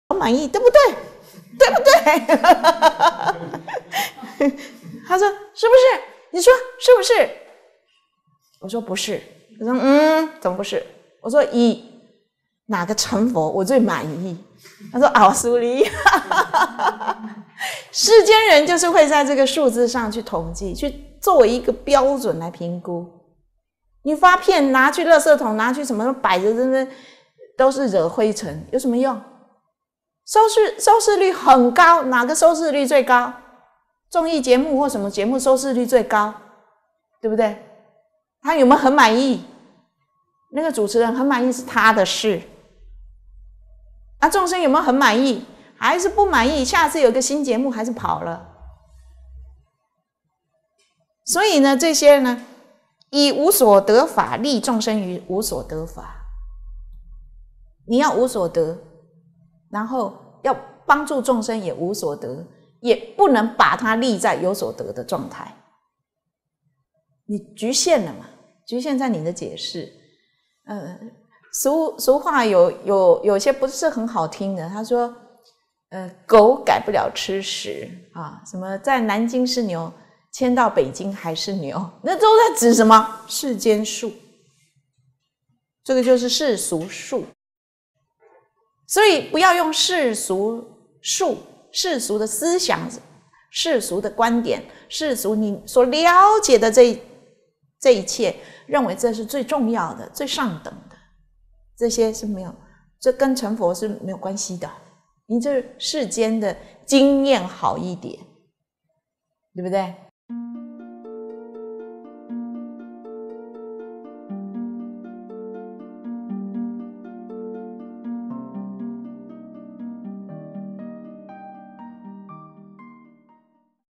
深入經藏 般若系列一 般若波羅蜜多心經 印可法師 主講 光碟5-12 財團法人嚴淨菩提教育基金會 製作發行
護持銀行帳戶:
銀行代號: 006
戶名: 財團法人嚴淨菩提教育基金會
帳號: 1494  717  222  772

地址︰基隆市七堵區南興路148號

300新竹市北區北大路346號13F之1